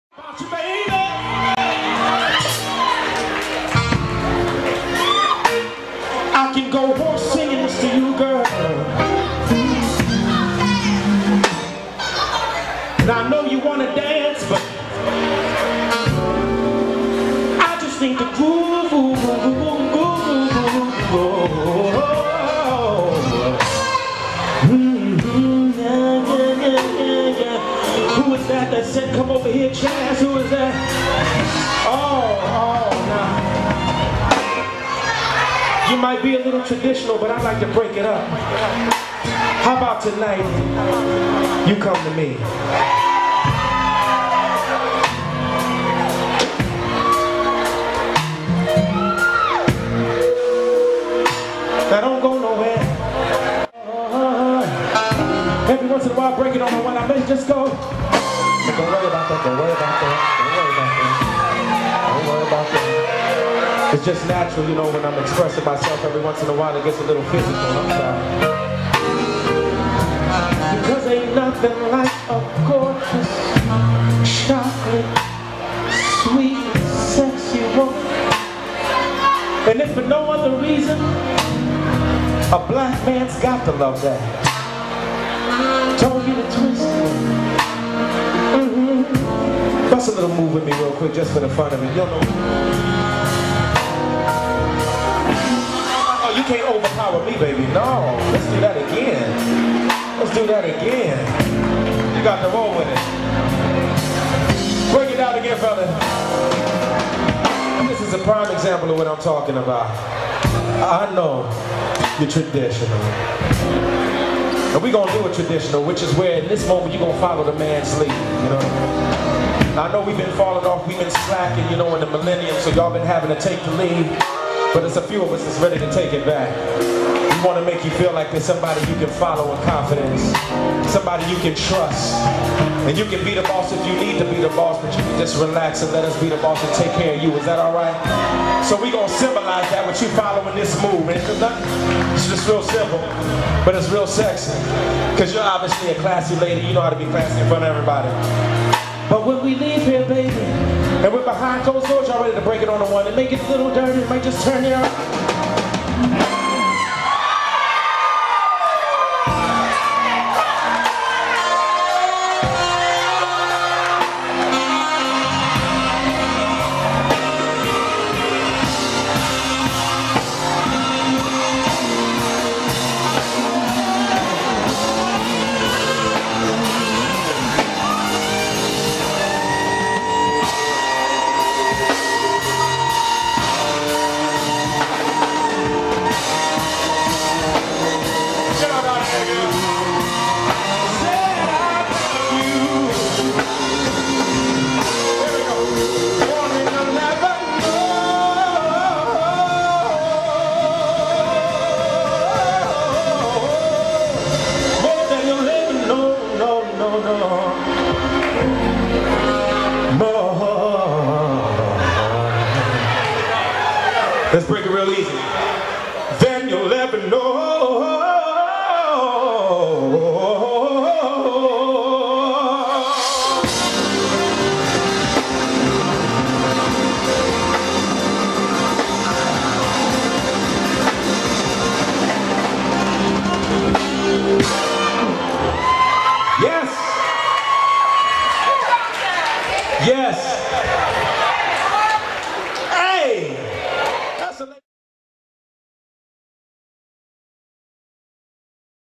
Chaz Shepherd at Buttons Addison Live
Chaz's ask's a young lady for a" DANCE"!